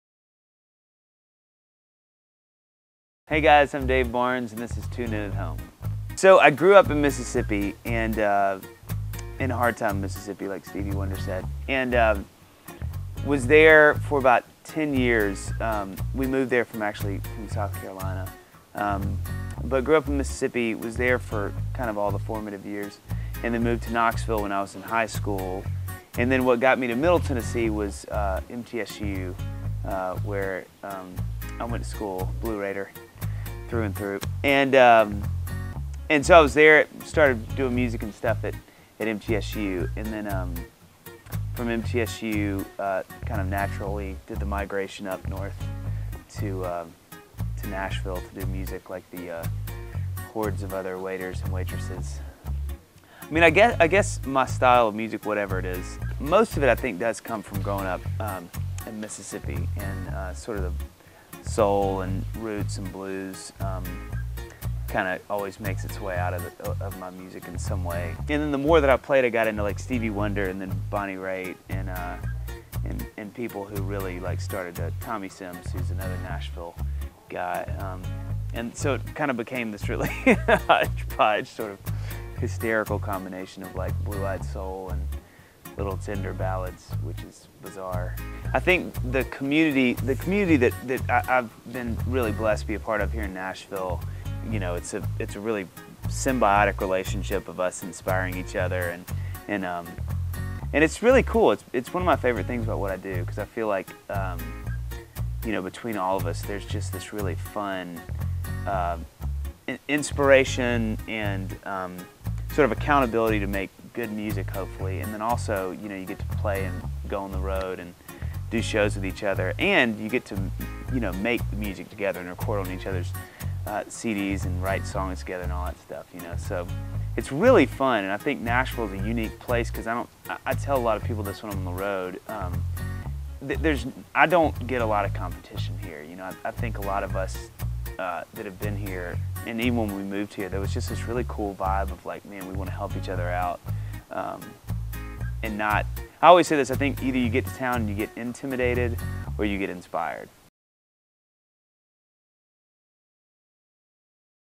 Tune In At Home - Dave Barnes - Introduction
Dave Barnes talks about his music and living in Nashville.